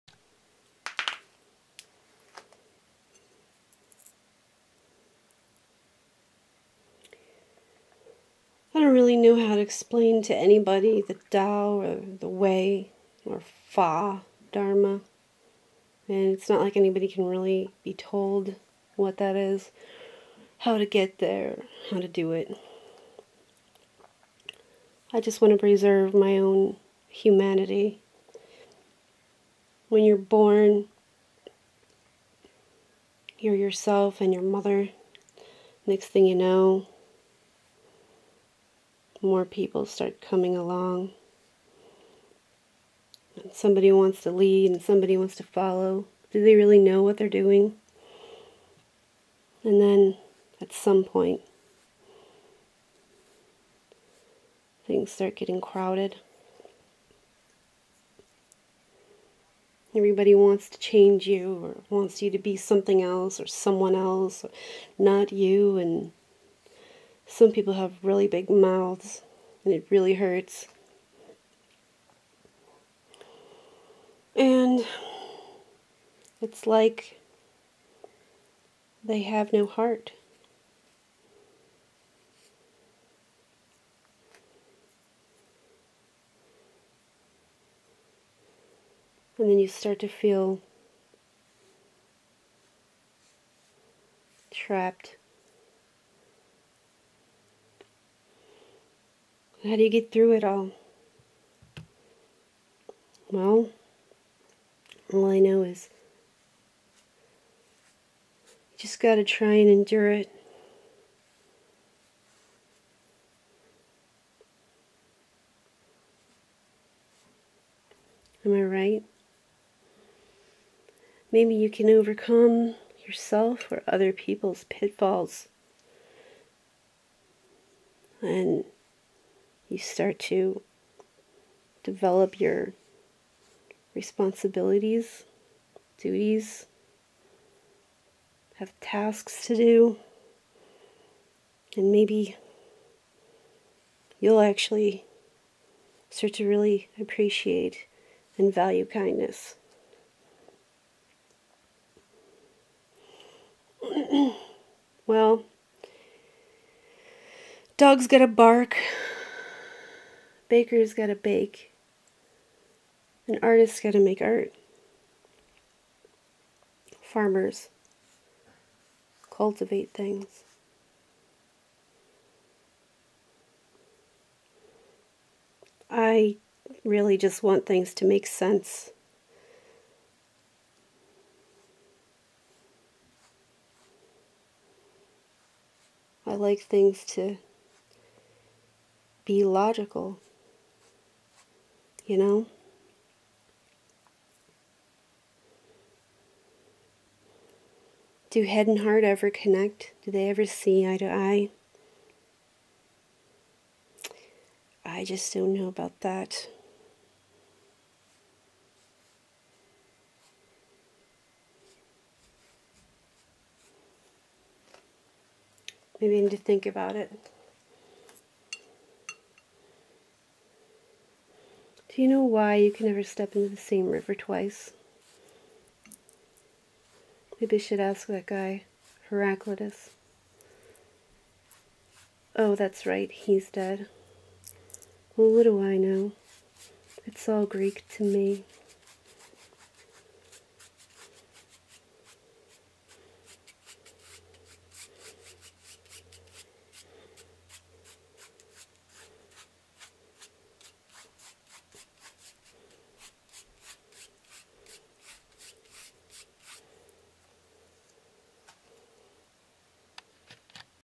Zi (Etymology Puns) Story [RESHOT] Uncut/Unedited
Uncut/Unedited
Probably the dozenth take! he to re-shoot!

I humbly apologize for my slacker-ish calligraphy of such low grade skills.

It's for my film project at UMASS Boston.